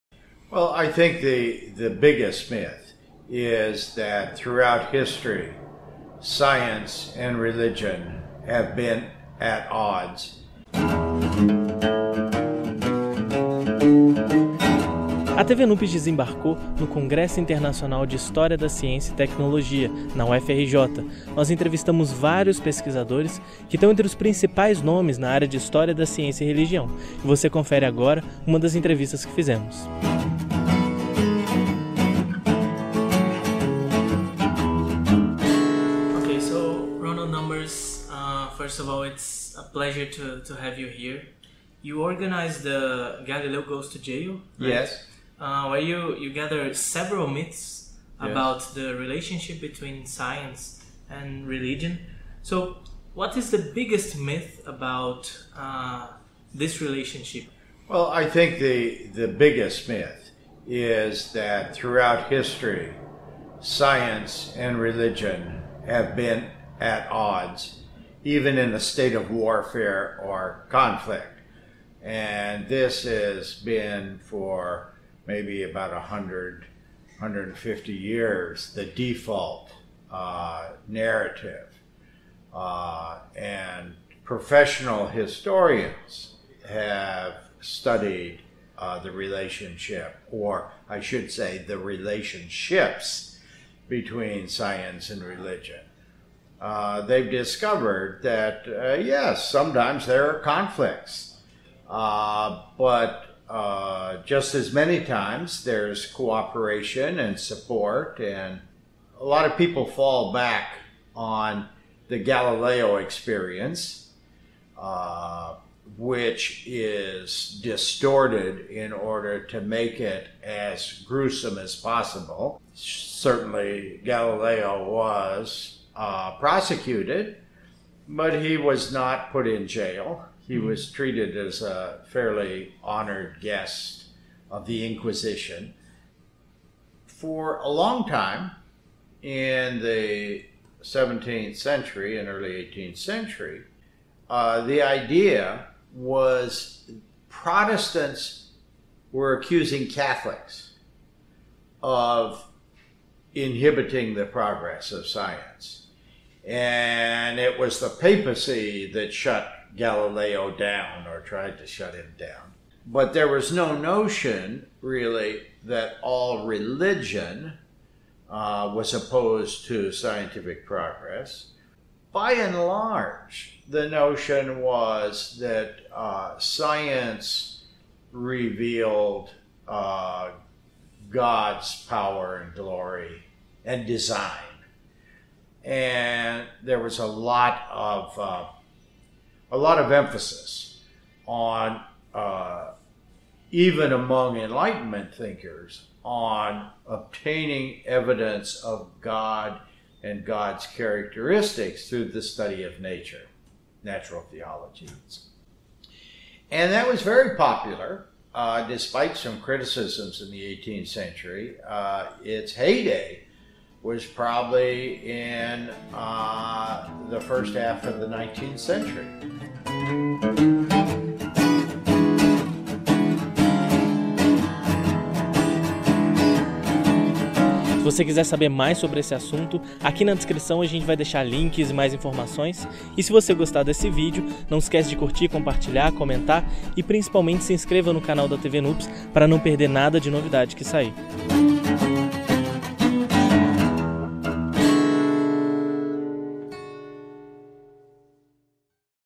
A relação histórica entre ciência e religião | Ronald Numbers (parte 1)
Ciência e religião sempre estiveram em conflito? Quais as principais caraterísticas dessa relação ao longo da história? Casos de conflito e de apoio mútuo. O caso Galileu. O que é teologia natural?

Prof. Ronald L. Numbers é historiador, professor de História da Ciência e da Medicina na Universidade de Wisconsin-Madison (EUA). Ele foi premiado com a Medalha George Sarton pela Sociedade de História da Ciência (EUA) em 2008. Autor de mais de 50 de artigos e 20 livros, principalmente sobre História da Ciência, História da Medicina e de suas relações com a religião, sendo um dos mais conhecidos: Galileu Goes to Jail and other Myths about Science and Religion (Harvard, 2009). Editor geral do Cambridge History of Science (2003-, 8 vol.). Ex-presidente da History of Science Society, da American Society of Church History e da International Union of History and Philosophy of Science.

Para saber mais:
Galileo Goes to Jail and Other Myths about Science and Religion. Ronald L. Numbers (Org.). Harvard University Press, 2009.

Mitos históricos sobre a relação entre ciência e religião (resenha)
Alexander Moreira-Almeida
Revista de Psiquiatria Clínica 36(6):252-3, 2009

Mitos e verdades em ciência e religião: uma perspectiva histórica
Ronald L. Numbers
Revista de Psiquiatria Clínica 36(6):246-51, 2009